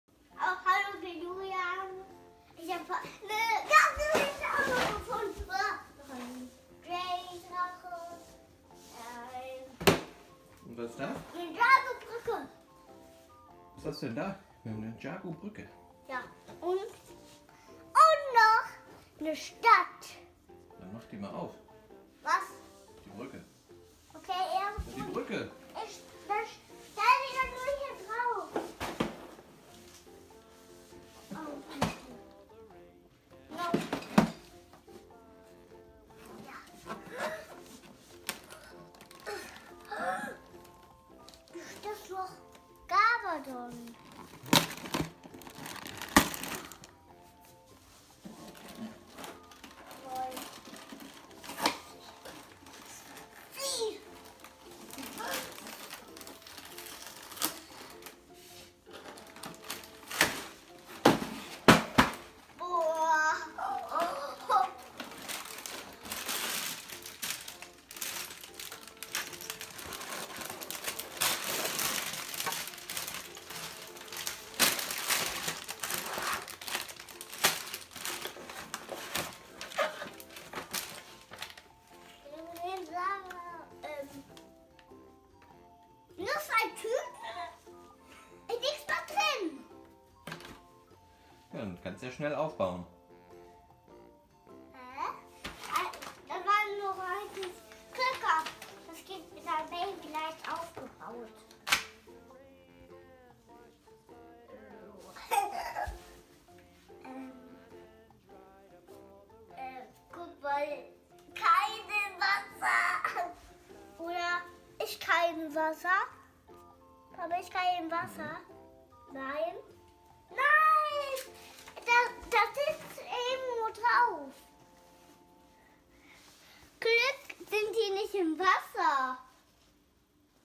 Ninjago Meister Wu's Wasser-Fall Spielzeugtester Julian
Das LEGO Ninjago 70608 – Meister Wu’s Wasser-Fall ist ein absoluter Klassiker und ein absolutes Muss für jeden Dschungelfan. Meister Wu´s Wasserfall ist voll von Überraschungen, wie beispielsweise einer verborgenen Höhle. Dabei kann die Holzbrücke mit zahlreichen anderen Dingen kombiniert werden. Aber auch als Einzelstück ist sie eine echte Besonderheit und kann in den unterschiedlichsten Szenarien verwendet werden. Hier können Kinder ihrer Kreativität und Fantasie freien Lauf lassen und immer wieder neue Geschichten erfinden. Ein Spielspaß der keine Grenzen kennt. Auch wenn es sich dabei eher um ein Spiel für Jungen handelt, so heißt dies noch lange nicht das tatsächlich auch nur Jungen daran Interesse finden. Ganz im Gegenteil. Immer mehr Mädchen interessieren sich für die Welt des Geheimnisvollen und Mystischen. Puppenspiele waren gestern. In der heutigen Zeit stehen die Fantasiewelten im Vordergrund.